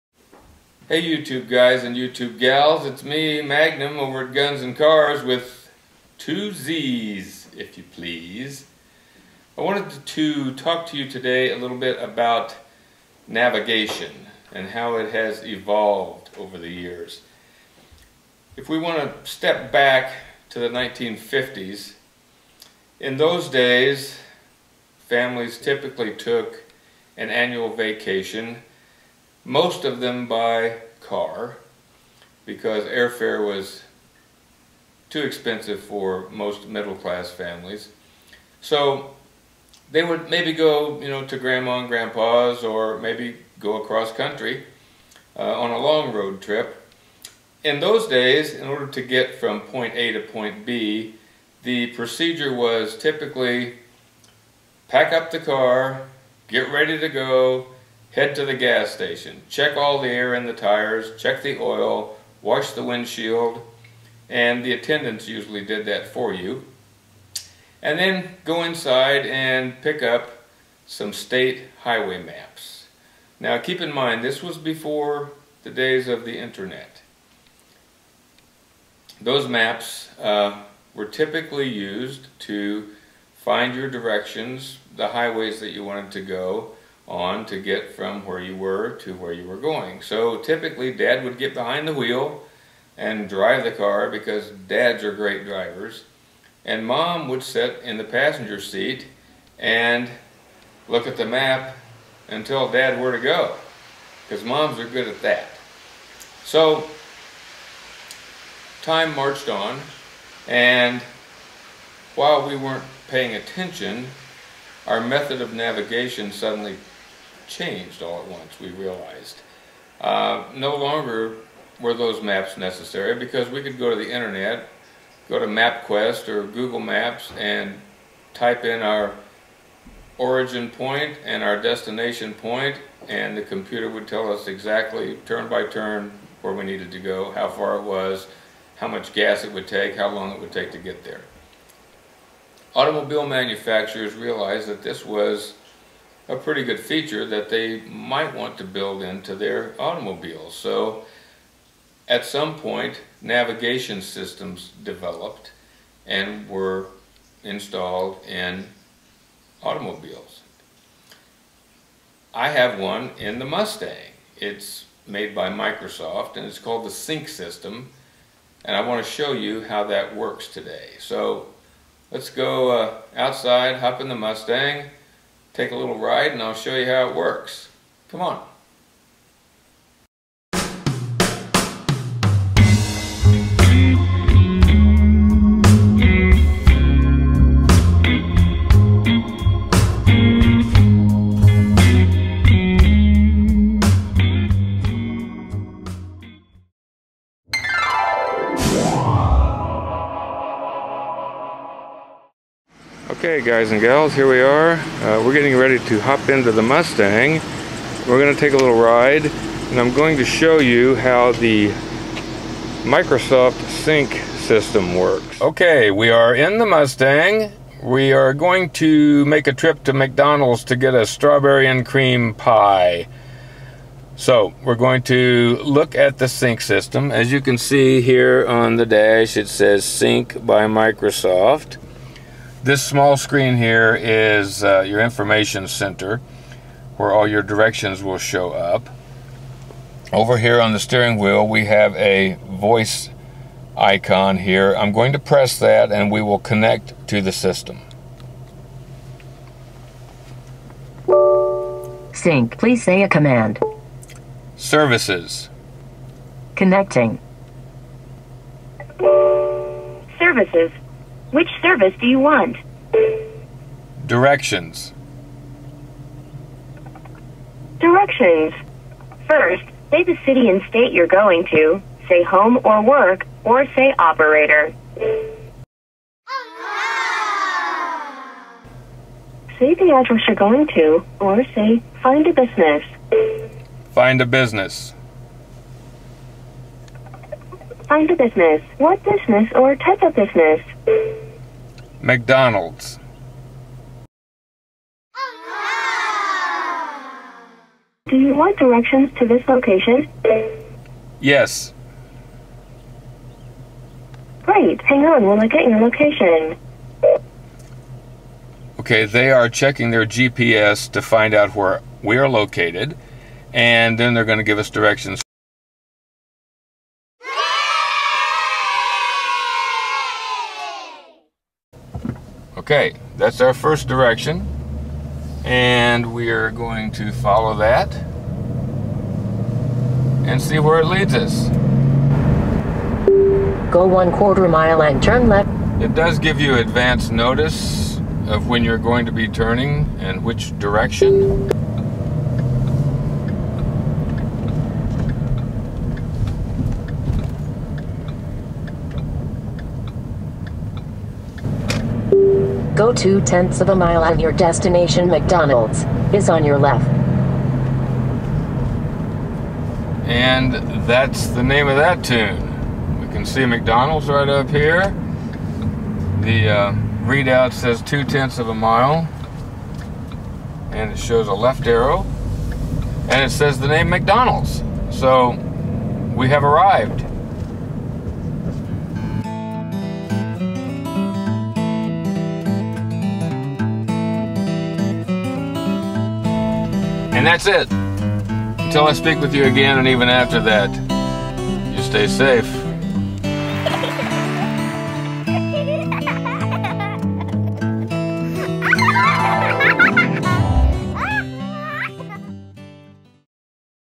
MUSTANG SYNC
This video gives an overview of the Mustang SYNC system by Microsoft.